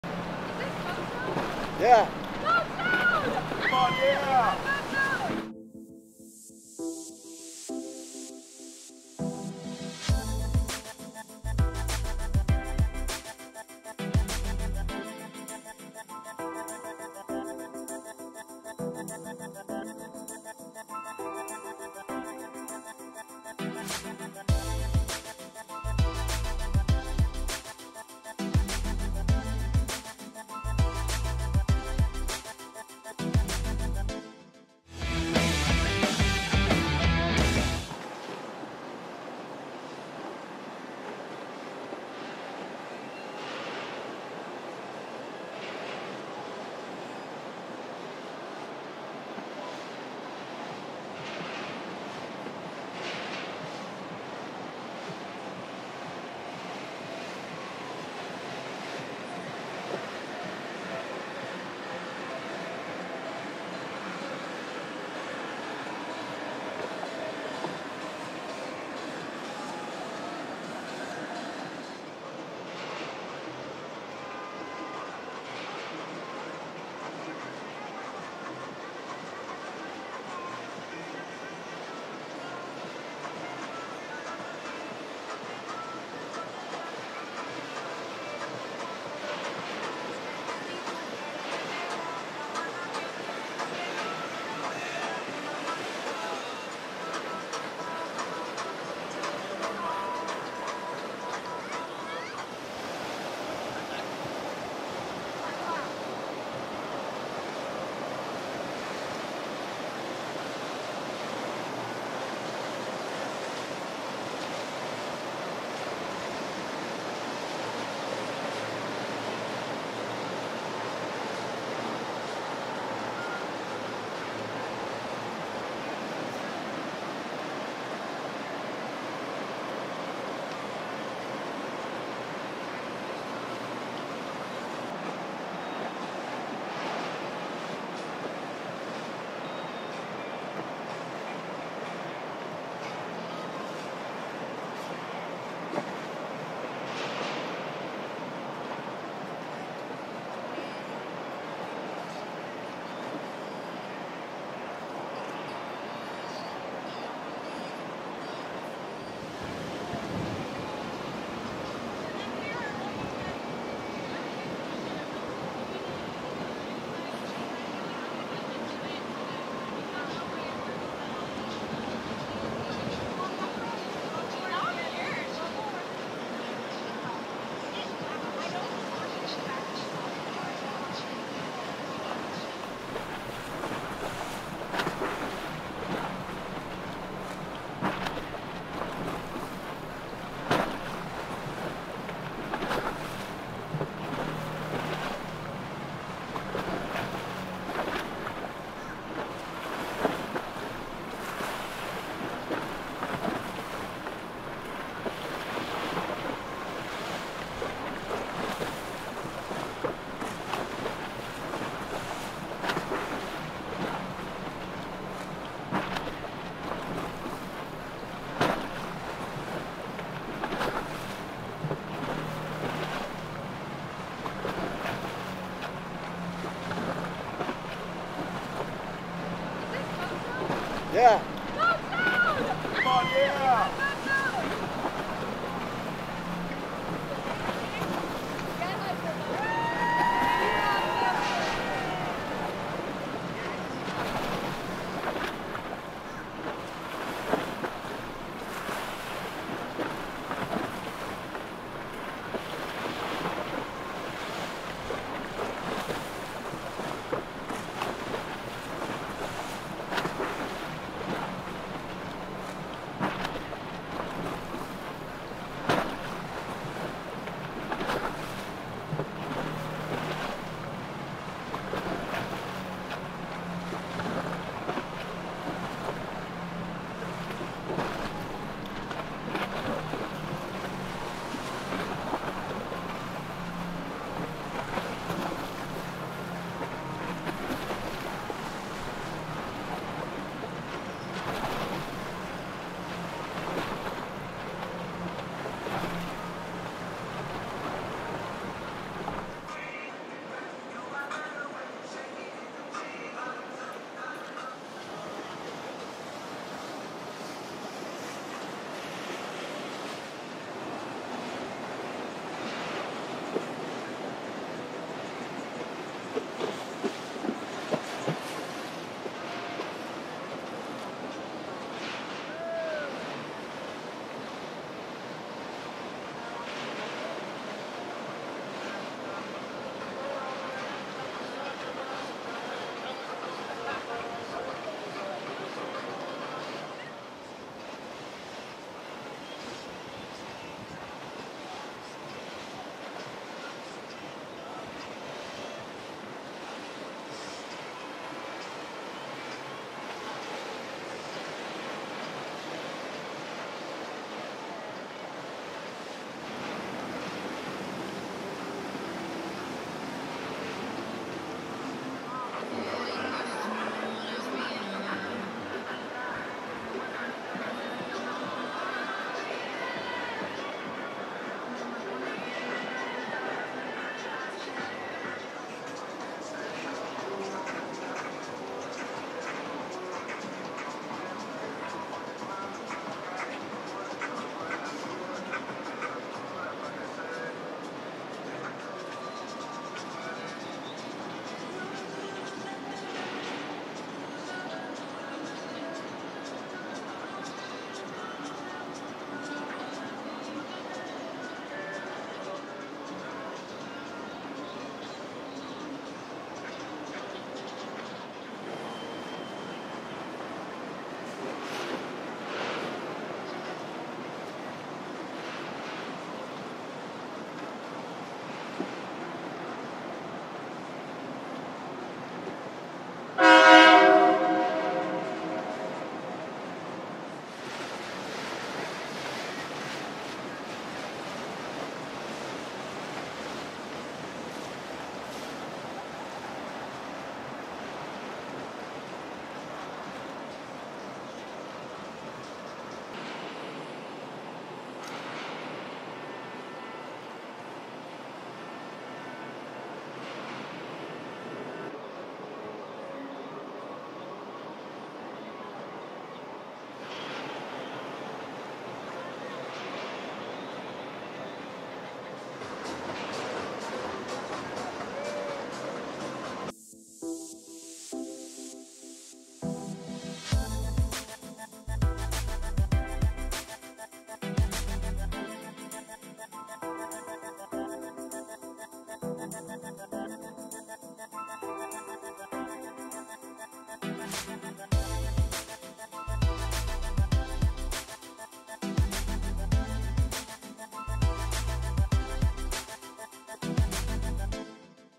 All Eyes on Her! Red or Black?
The Miami River is a popular party spot known for its super yachts, sexy girls, crazy boat parties, and spring breakers. It's a hub for boating enthusiasts and offers both private and organized charters for an unforgettable experience.